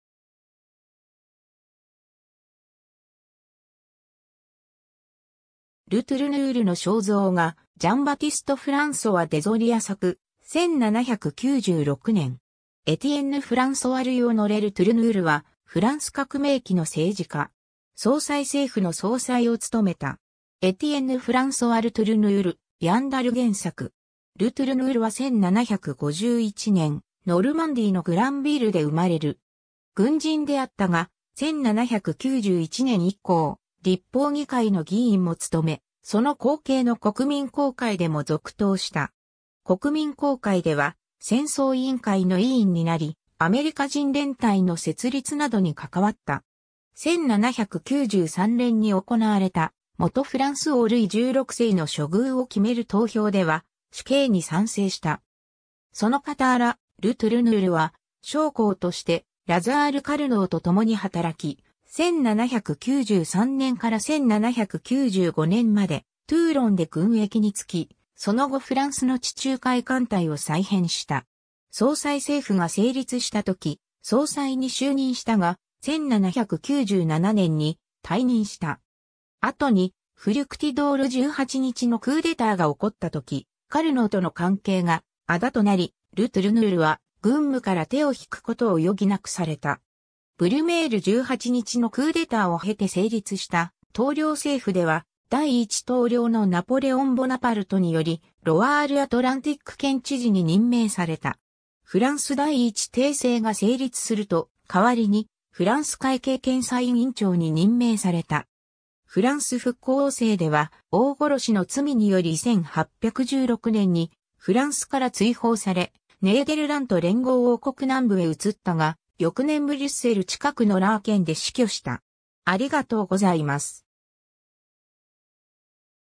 エティエンヌ＝フランソワ・ル・トゥルヌール
ル・トゥルヌールの肖像画、ジャン＝バティスト・フランソワ・デゾリア作、1796年。
 エティエンヌ＝フランソワ＝ルイ＝オノレ・ル・トゥルヌール（フランス語: Étienne-François-Louis-Honoré Le Tourneur、1751年3月15日 - 1817年10月4日）は、フランス革命期の政治家。
総裁政府の総裁（在任：1795年11月1日 - 1797年5月20日）を務めた。
 エティエンヌ＝フランソワ・ル・トゥルヌール、ヤン・ダルゲン作。
 ル・トゥルヌールは1751年、ノルマンディーのグランヴィルで生まれる。
軍人であったが、1791年以降立法議会の議員も務め、その後継の国民公会でも続投した。
国民公会では戦争委員会の委員になり、アメリカ人連隊の設立などに関わった。
1793年に行われた元フランス王ルイ16世の処遇を決める投票では死刑に賛成した。
 その傍ら、ル・トゥルヌールは将校としてラザール・カルノーとともに働き、1793年から1795年までトゥーロンで軍役に就き、その後フランスの地中海艦隊を再編した。
総裁政府が成立したとき、総裁に就任したが、1797年に退任した。
後にフリュクティドール18日のクーデターが起こった時、カルノーとの関係が仇となり、ル・トゥルヌールは軍務から手を引くことを余儀なくされた。
ブリュメール18日のクーデターを経て成立した統領政府では第一統領のナポレオン・ボナパルトによりロワール＝アトランティック県知事に任命された。
フランス第一帝政が成立すると、代わりにフランス会計検査院院長に任命された。
 フランス復古王政では王殺しの罪（ルイ16世の処刑に賛成した）により1816年にフランスから追放され、ネーデルラント連合王国南部（のちのベルギー）へ移ったが、翌年ブリュッセル近くのラーケンで死去した。